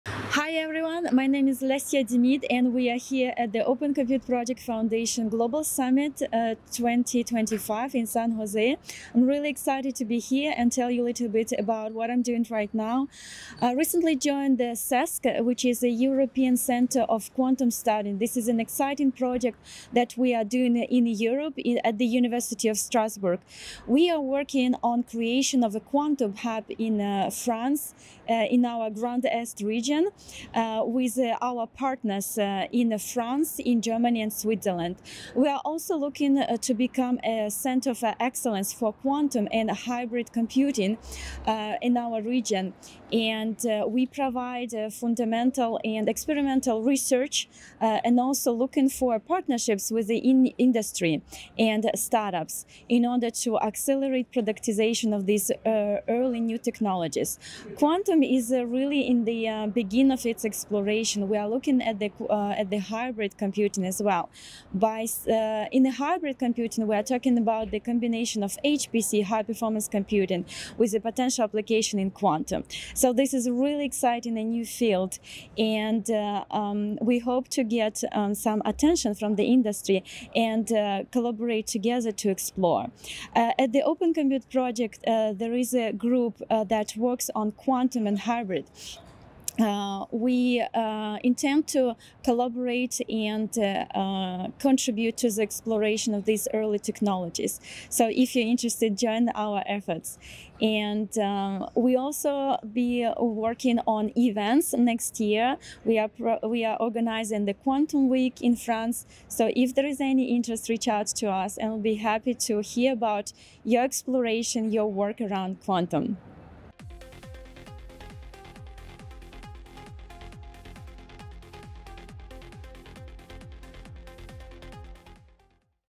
OCPSummit25: Building Europe's Quantum Hub
to get the attendee and sponsor perspectives from the show floor of the OCPGlobalSummit 2025

*Quantum Computing Takes Center Stage: European Research Hub Explores Hybrid Technologies*

Lesya Dymyd, Business Development Lead at the Centre Européen de Sciences Quantiques (CESQ), shares insights from the OCP Global Summit 2025 about groundbreaking developments in quantum computing research. Speaking from San Jose, Dymyd discusses her new role at CESQ, a European quantum research center, and reveals ambitious plans for establishing a quantum excellence hub spanning multiple countries.

- How European institutions are building cross-border quantum research partnerships
- The emerging field of hybrid computing that combines HPC with quantum applications
- Strategic collaborations between academic research centers and industry partners
- Upcoming opportunities for quantum technology exploration and networking
- The role of Open Compute Project's quantum group in advancing early-stage technologies

Dymyd outlines the center's mission to accelerate quantum technology productization through industry partnerships and reveals plans for a major quantum event in France. This conversation provides valuable insights into the current state of quantum research and the collaborative efforts shaping its future.